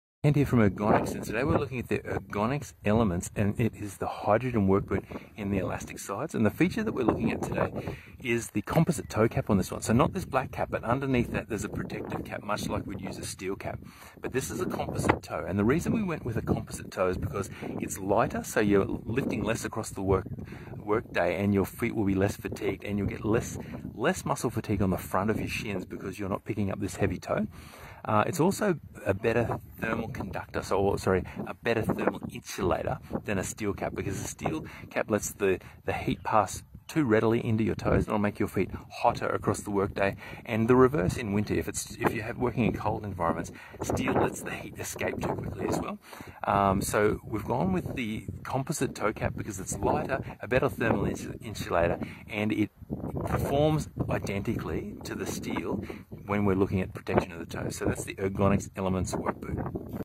The Ultimate Comfort: Composite Toe Arch Support Safety Boots in Australia
If you're on the hunt for the most comfortable safety boots in Australia, look no further! In this video, we explore the exceptional comfort and support of composite toe arch support safety boots. Say goodbye to foot fatigue and discomfort as we showcase the outstanding benefits of these boots equipped with Ergonx technology and orthotics. Our expert analysis highlights why these work boots stand out from the rest, providing unparalleled comfort and protection for your feet. Whether you work in demanding environments or enjoy outdoor activities, these composite toe safety boots are a game-changer!

Ergonx / Docpods is a foot pain and foot health company run by an Australian podiatrist.

For more information please visit:

Ergonx:  Ergonx.com.au
Docpods: Docpods.com.au